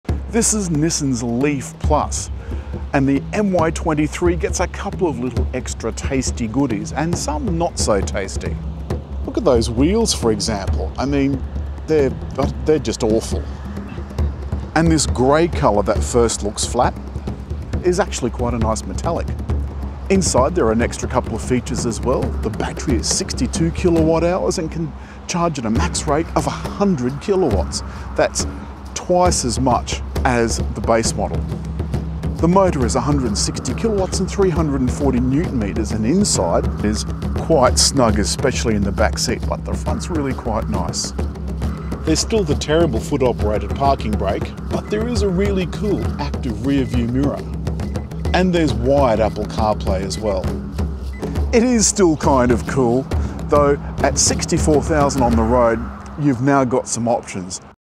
2023 Nissan Leaf + 1 Min Video Review
Nissan launched the leaf, and while we liked it, we thought it was a little lacking, range-wise. Then quick as a flash, Leaf + came along. The base model remains, but Leaf + increases the 40kWh battery to 62kWh, and the power from 110kw/320Nm to 160kw/340Nm. Maximum charging rate is up from 50kw to 100kw, but only the top model, with range now between 300 and 400km, or thereabouts.
Nissan Leaf remains one of the only EV’s with bi-directional VTG charging. In other words, with appropriate infrastructure, can be used as a mobile battery at home, or when out at a public charger. The Hyundai Group EVs have VTL, or, Vehicle to load. It enables their electric cars to power anything plugged into one of the 2 household-style power sockets (1 under the back seat and the other on the external adaptor that plugs into the charge port).
That’s a whole heapin’ helpin’ of juicy electronic goodness.
It changed the way I felt about the little EV, despite the criminally ugly wheels. The MY 23 Leaf + still has 17” alloys, but now have a weird pentagonal lined pattern in them that makes them look like board game tokens.